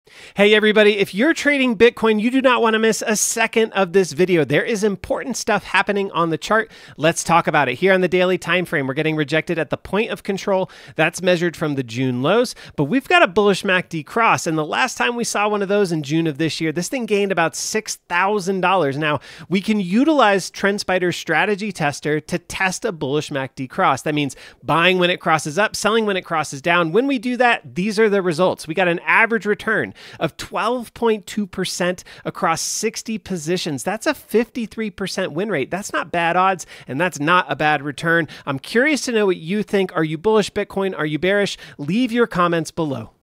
$BTC BTC Bullish MACD Cross!
There's a bullish MACD cross on the $BTC daily chart! Is it poised for another move higher? Let's see!